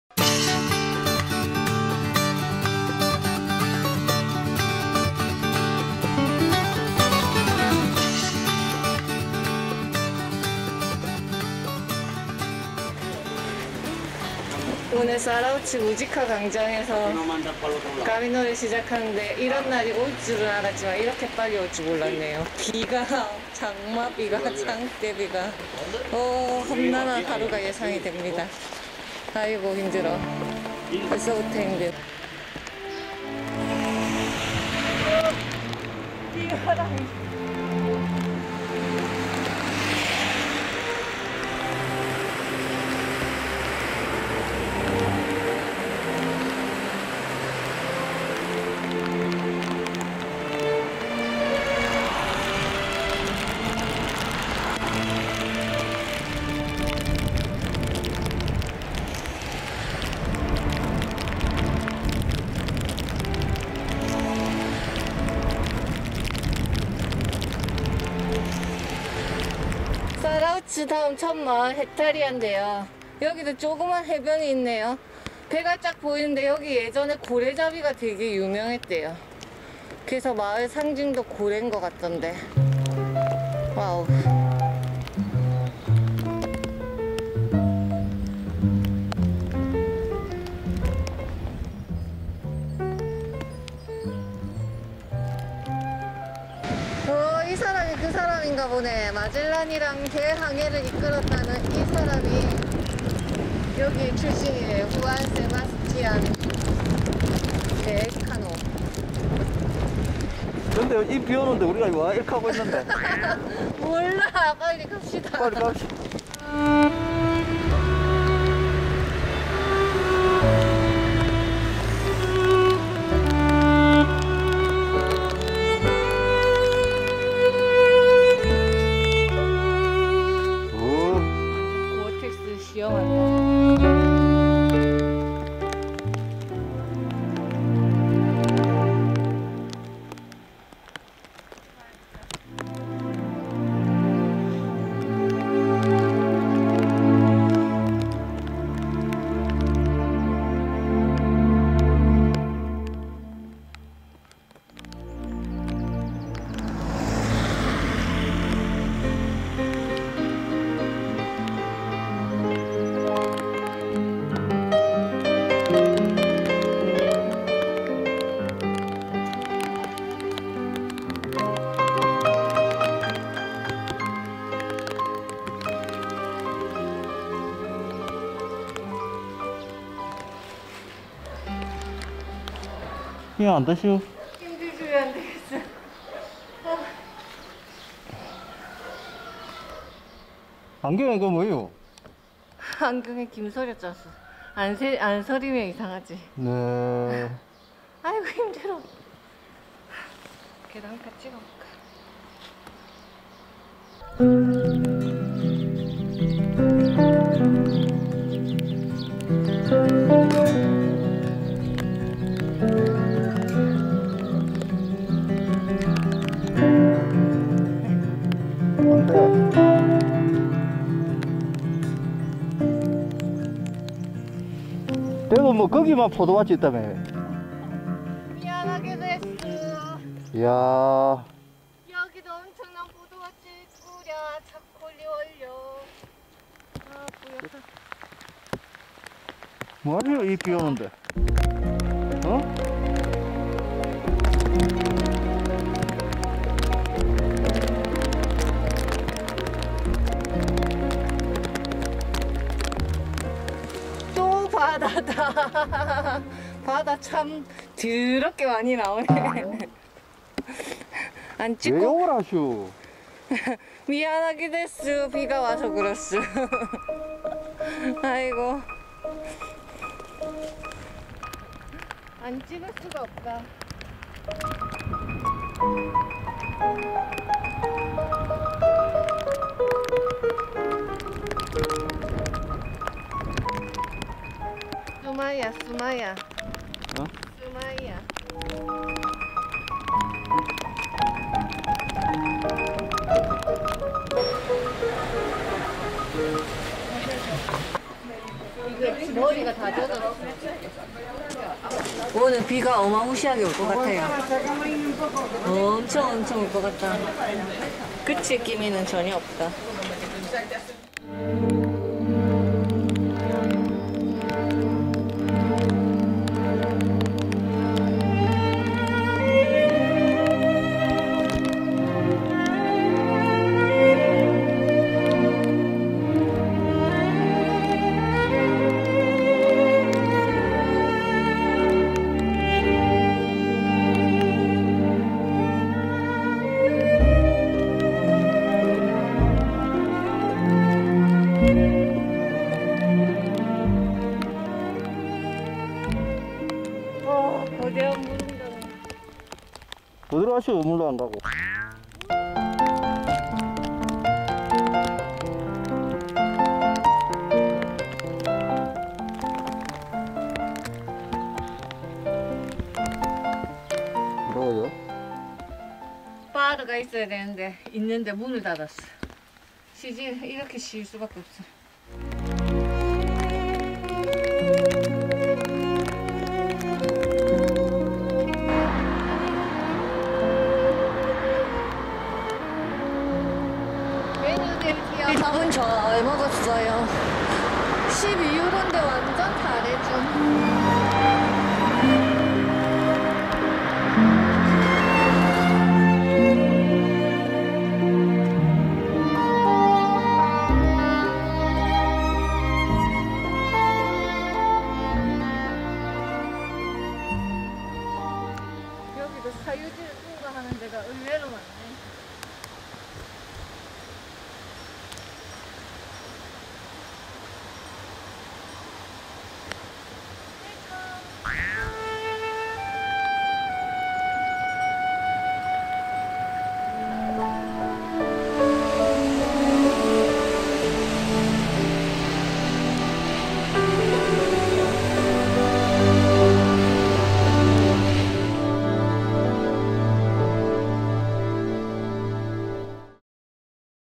BigFoot 산티아고 북쪽길 순례 7회 (사라우츠~이치아르/Zarautz~Itziar/18.53km) El Camino Norte de Santiago 7
산티아고 북쪽 순례길 전체 여정을 담은 최초 동영상!
그 일곱번째 영상으로 사라우츠에서 이치아르까지 18.53km를 걷는 여정을 담았습니다.
순례 시작부터 몰아친 비바람이 하루 여정을 마칠 때까지 그치지 않아 정말 힘겨웠어요. 하지만 함께 걷는 순례자들이 있어 힘을 얻었고 넓게 펼쳐진 초원과 포도밭이 위로가 됐으며 북쪽길 내내 함께 걸어준 바다 또한 친근함으로 응원을 해줬답니다.

The 7th film of El Camino Norte de Santiago.
Zarautz ~ Itziar, 18.53km.